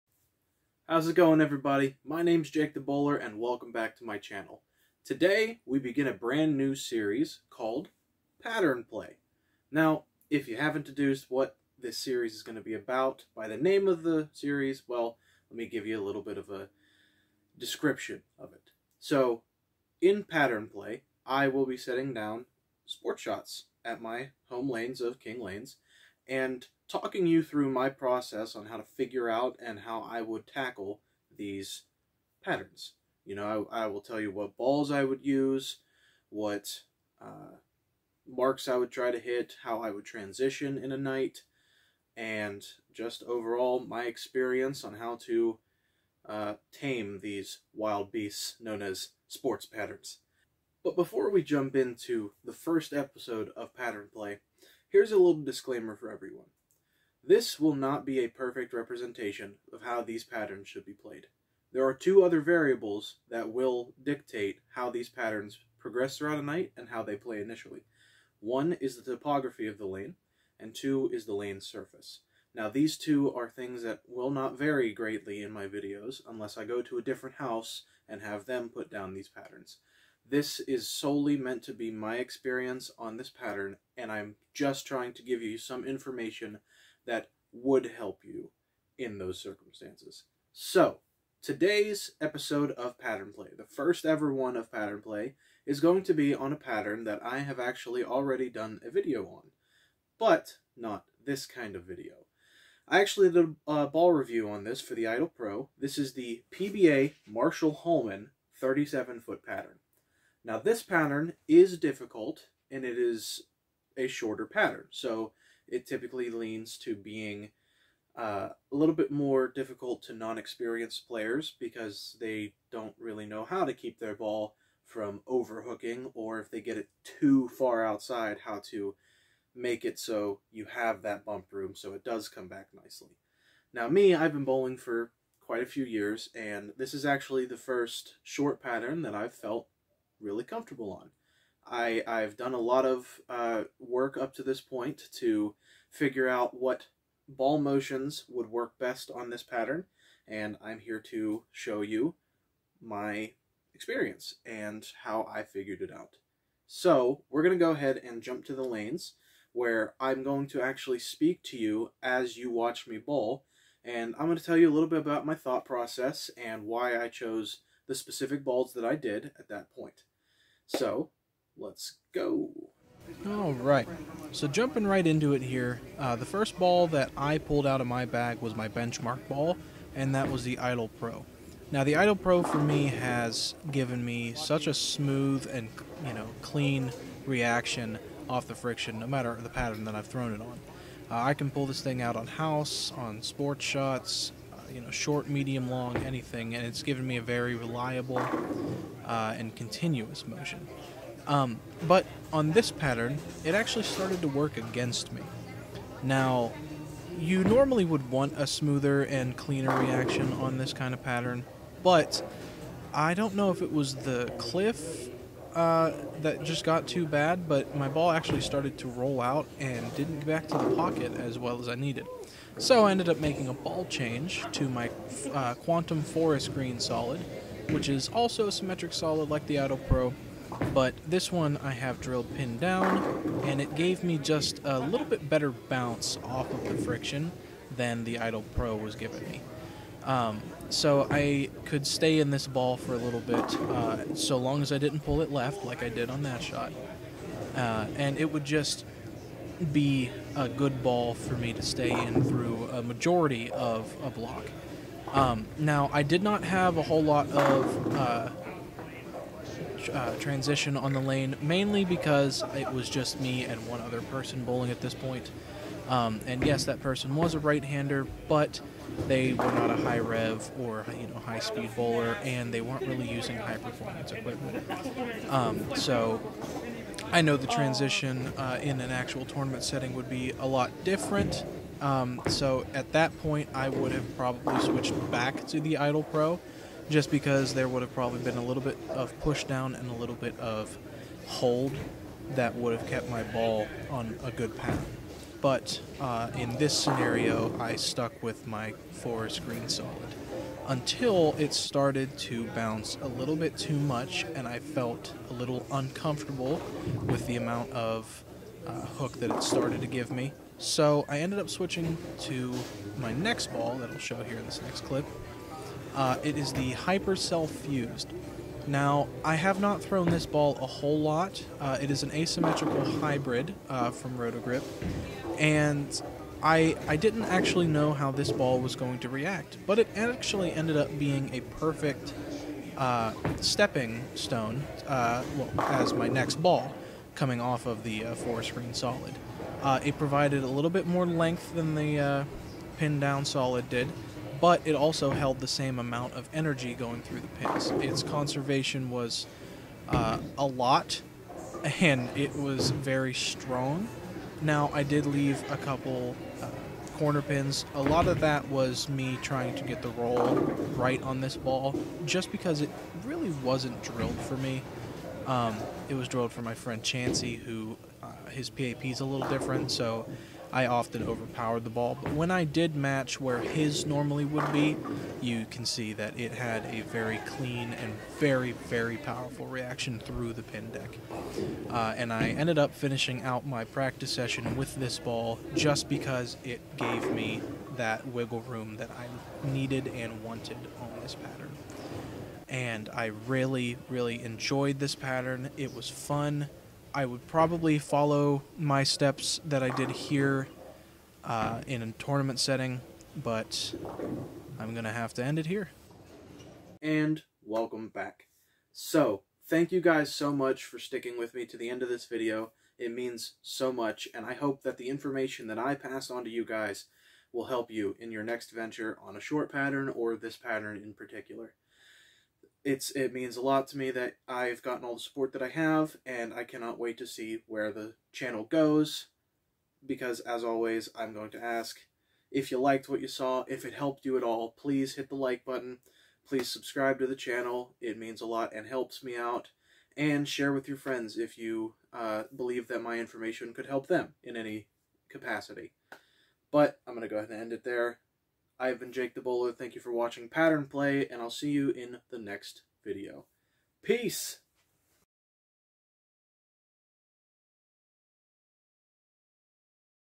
PBA Marshall Holman 37' - Pattern Play Episode 1
Good day everyone! Welcome back to the channel! In todays video, I talk you through my experience on the 37' Marshall Holman PBA oil pattern. As part of this new series, you will see my personal adjustments and plans regarding this pattern and its transition!